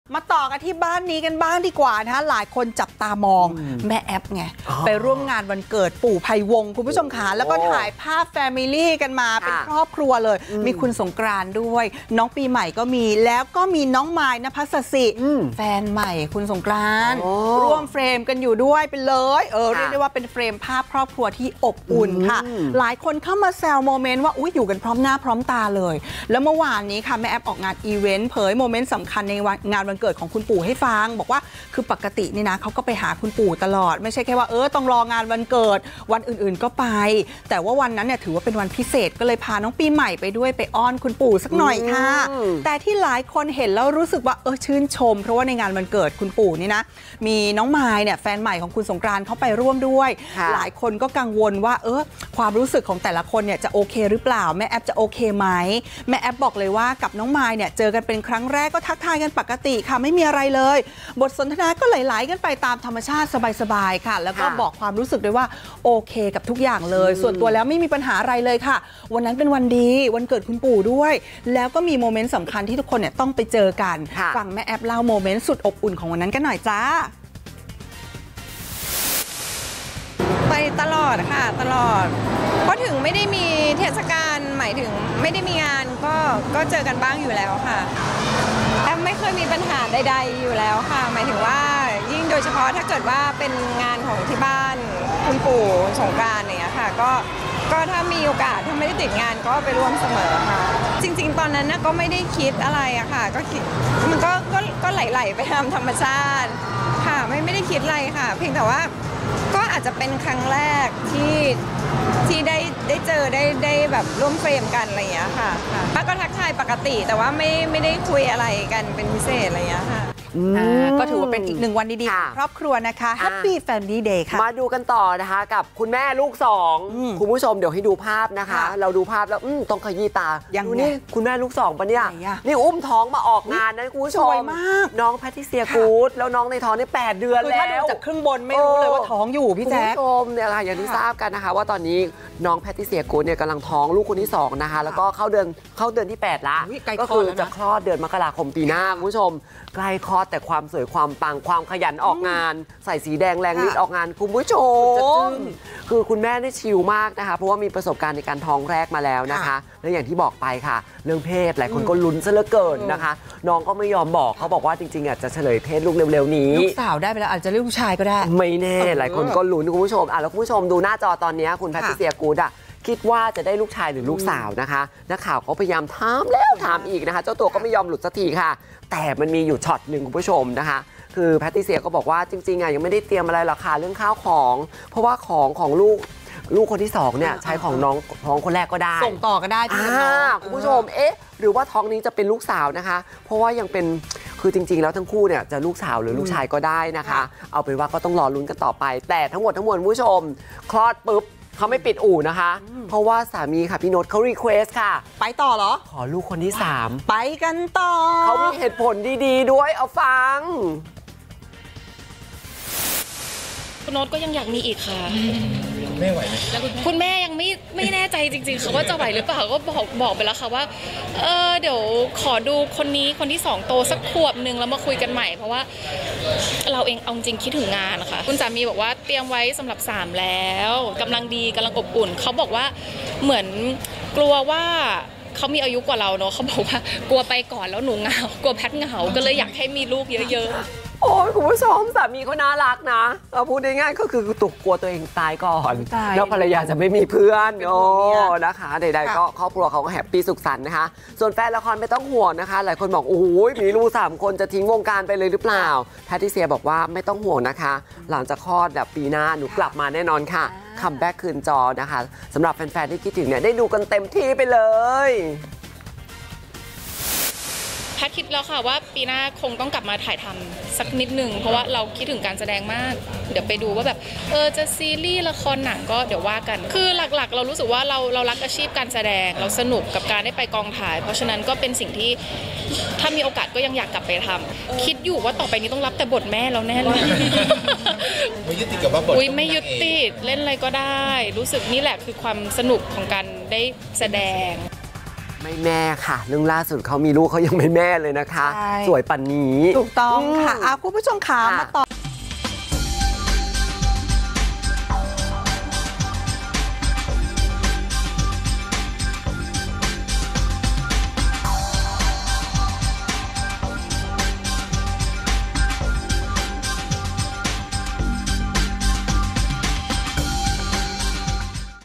"แอฟ" เล่าโมเมนต์อบอุ่นร่วมเฟรมภาพ "สงกรานต์-มายด์" | 27 พ.ย. 67 | บันเทิงไทยรัฐ
ข่าวนี้ทำเอาหลายคนจับตามองหลังจากที่แม่แอฟ ไปร่วมงานวันเกิดคุณปู่ ไพวงศ์ และถ่ายภาพครอบครัวพร้อมหน้าพร้อมตา ทั้งคุณสงกรานต์ น้องปีใหม่ แถมยังมีสาวมายด์ ณภศศิ ร่วมเฟรมอีกด้วย